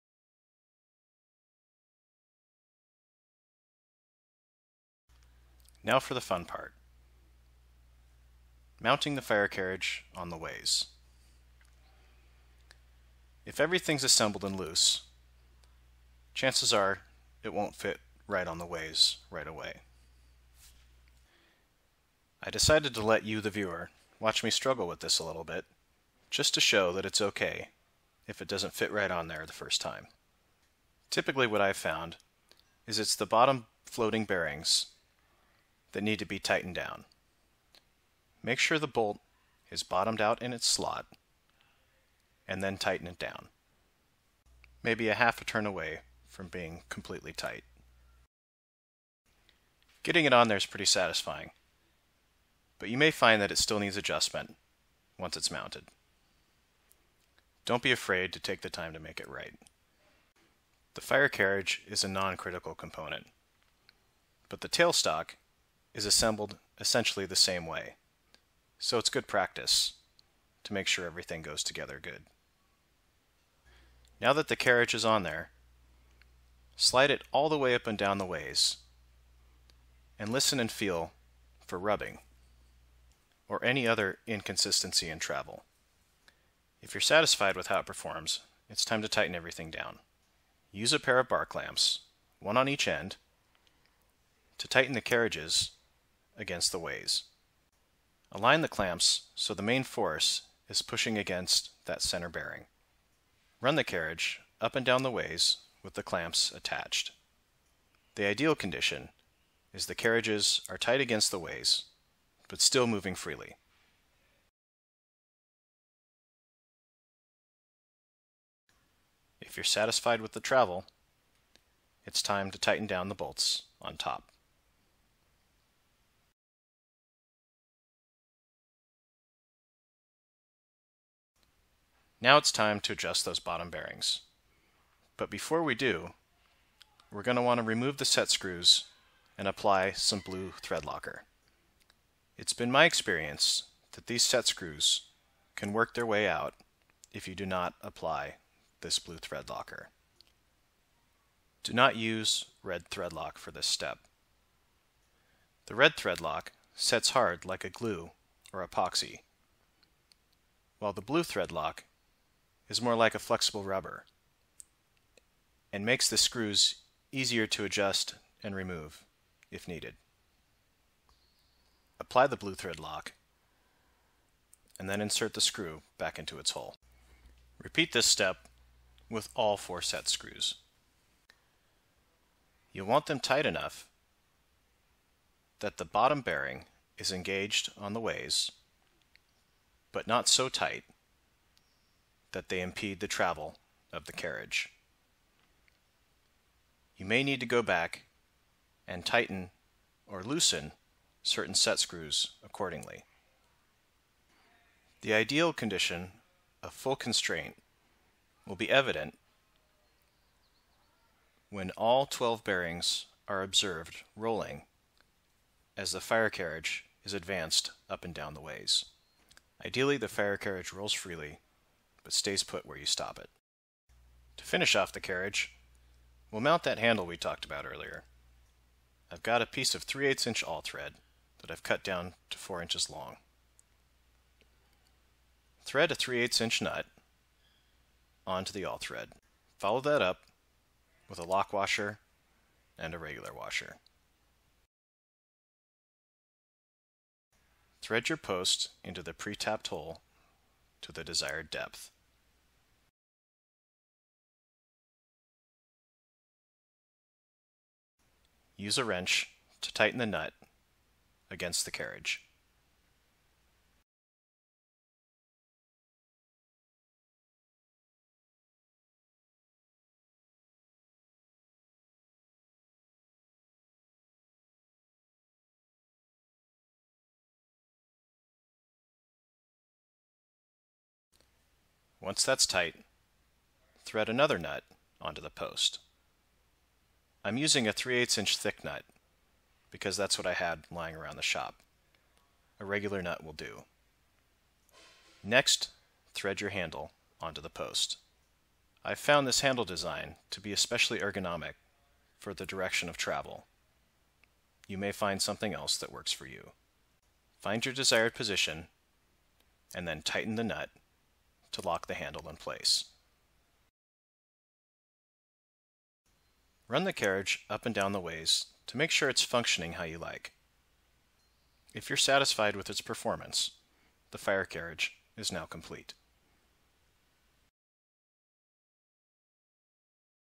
Fire carriage 2
This is the second video in the fire carriage series. I show how to adjust and mount the carriage as well as install the handle.

Additional carriage assembly and adjustment instructions from the manufacturer can be found here:

Parts list:
Qty: 1
Part # 6477K72
Description: Two arm knob 3/8"-16 Threaded Insert, 2 - 1/4" Diameter, Polypropylene
Supplier: McMaster- Carr
Each: $2.35 Total: $2.35

Additional parts for handle - Hardware store:
2 - 3/8" -16 nuts
2 - 3/8" -16 washers
2 - 3/8" -16 lock washers
4" 3/8" -16 threaded rod

This is the thread lock I like to use. It's more of a gel so it sticks to the threads better. The typical locktight is runny and can get all over you if you're doing small parts.